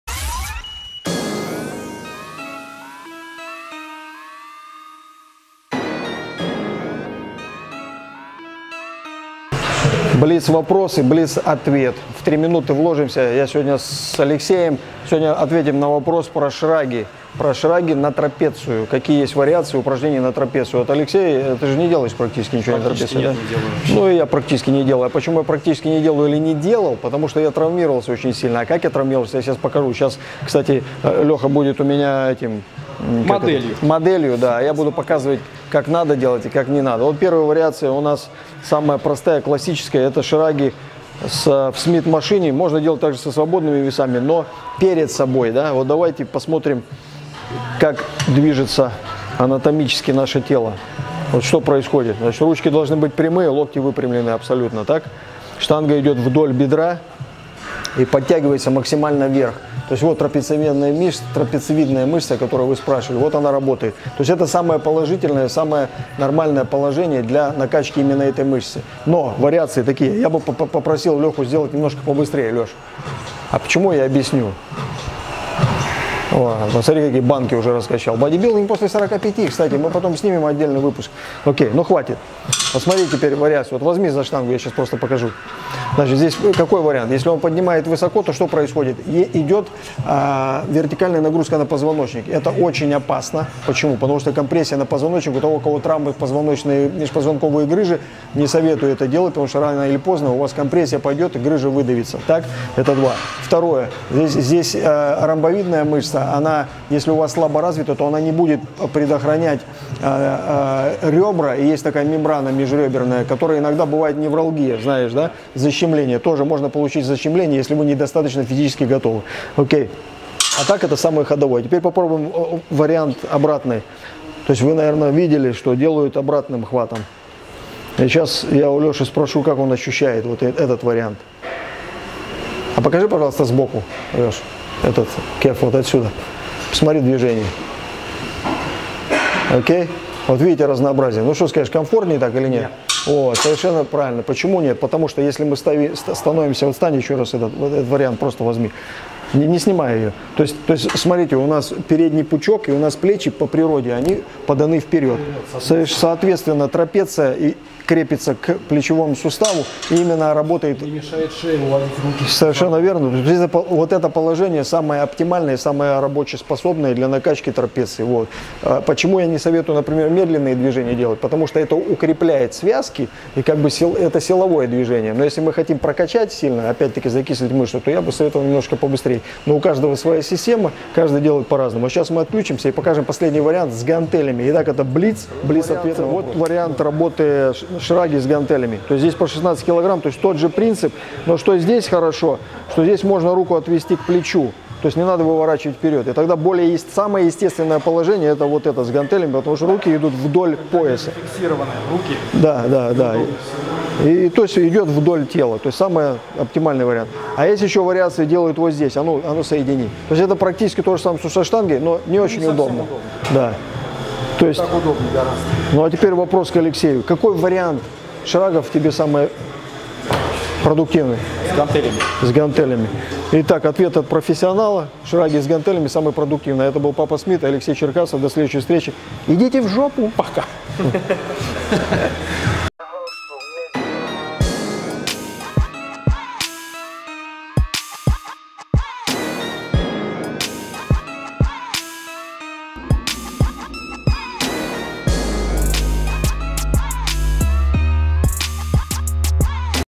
ТРАПЕЦИЯ - ПРАВИЛЬНАЯ ТЕХНИКА ОТ ПАПЫ СМИТА
Мастре спорта по бодибилдингу, силовой и кондиционный тренер Олимпийской сборной Китая, персональный тренер Владислав Кузнецов ака Папа Смит и его старый друг мастер спорта по бодибилдину Алексей Черкасов показывают самые продуктивные упражнения на трапецию!